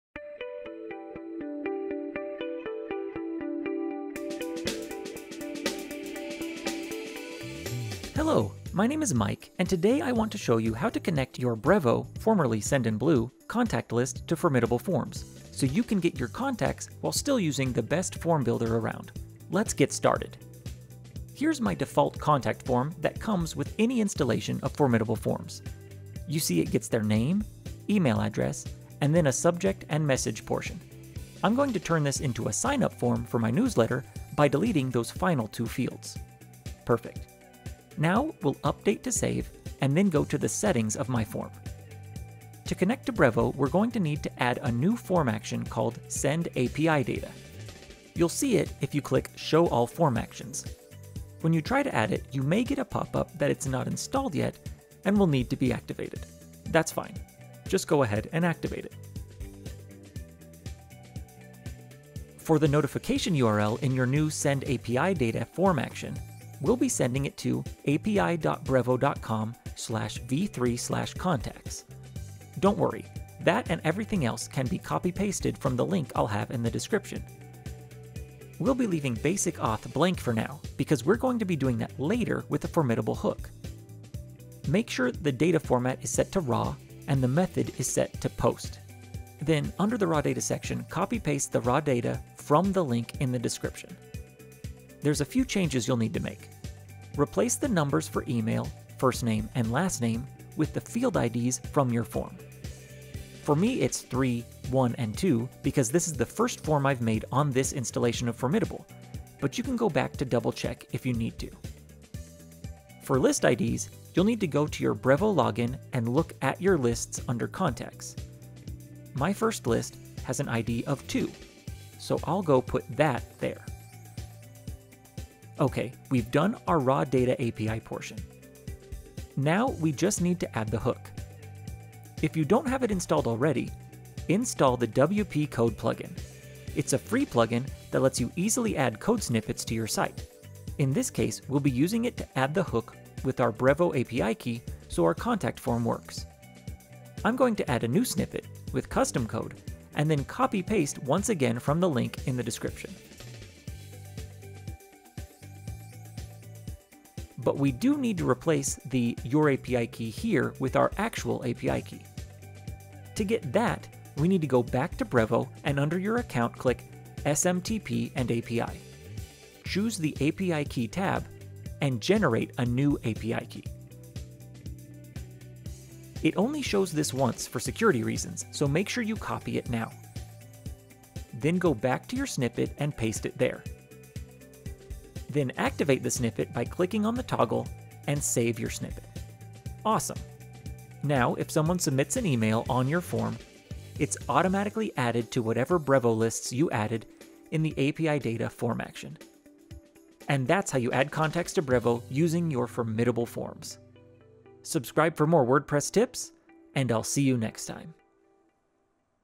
How to Connect Your Forms to Brevo
Connect your Formidable Forms to Brevo (formerly SendinBlue) with a quick form action and hook

0:00 - Intro
0:42 - API Form Action
2:08 - API Hook (& Key)